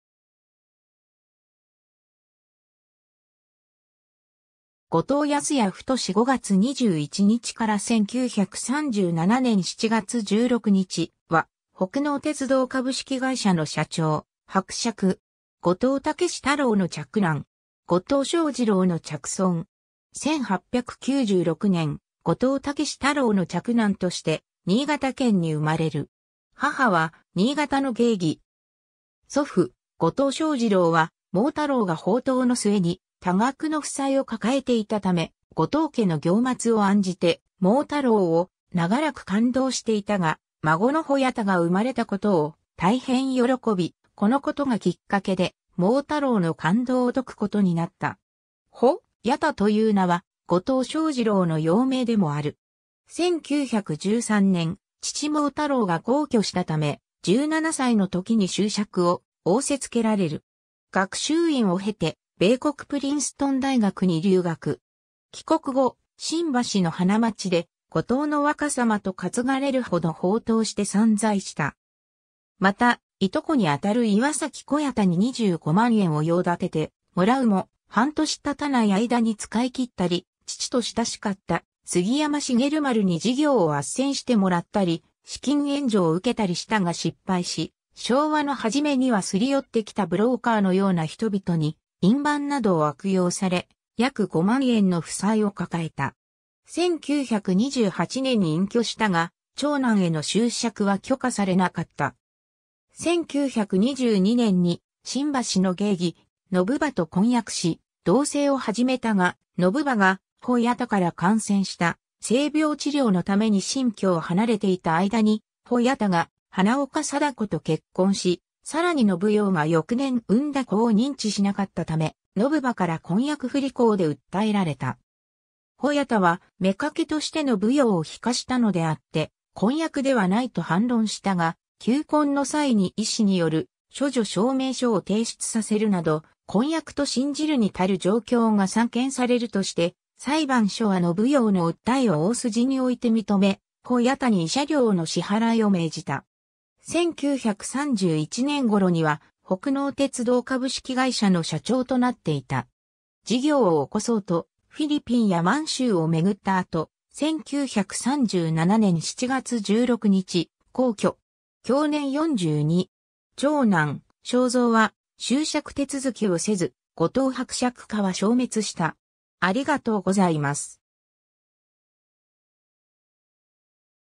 後藤保弥太
後藤 保弥太（ごとう やすやた、1896年（明治29年）5月21日 - 1937年（昭和12年）7月16日）は、北濃鉄道株式会社の社長、伯爵。
後藤猛太郎の嫡男。
後藤象二郎の嫡孫。
 1896年（明治29年）、後藤猛太郎の嫡男として、新潟県に生まれる。
母は新潟の芸妓。
 祖父後藤象二郎は、猛太郎が放蕩の末に多額の負債を抱えていた為、後藤家の行末を案じて、猛太郎を永らく勘当していたが、孫の保弥太が生まれたことを大変喜び、このことがきっかけで、猛太郎の勘当を解くことになった。
「保弥太」という名は、後藤象二郎の幼名でもある。
 1913年（大正2年）、父猛太郎が薨去したため、17歳の時に襲爵（伯爵）を仰せ付けられる。
 学習院を経て、米国プリンストン大学に留学。
帰国後、新橋の花街で「後藤の若様」と担がれるほど放蕩して散財した。
また、いとこに当たる岩崎小弥太に25万円を用立ててもらうも半年経たない間に使い切ったり、父と親しかった杉山茂丸に事業を斡旋してもらったり資金援助を受けたりしたが失敗し、昭和の初めには擦り寄ってきたブローカーのような人々に印判などを悪用され、約5万円の負債を抱えた。
1928年（昭和3年）に隠居したが、長男への襲爵は許可されなかった。
 1922年に新橋の芸妓・のぶ葉と婚約し同棲を始めたが、のぶ葉が保弥太から感染した性病治療のために新居を離れていた間に保弥太が花岡貞子と結婚し、さらにのぶ葉が翌年生んだ子を認知しなかったため、のぶ葉から婚約不履行で訴えられた。
保弥太は妾としてのぶ葉を落籍したのであって婚約ではないと反論したが、求婚の際に医師による処女証明書を提出させるなど、婚約と信じるに足る状況が散見されるとして、裁判所はのぶ葉の訴えを大筋において認め、保弥太に慰謝料の支払いを命じた。
 1931年（昭和6年）頃には北濃鉄道株式会社の社長となっていた。
 事業を興そうとフィリピンや満州を巡った後、1937年（昭和12年）7月16日薨去。
享年42。
長男・省三は襲爵手続きをせず、後藤伯爵家は消滅した。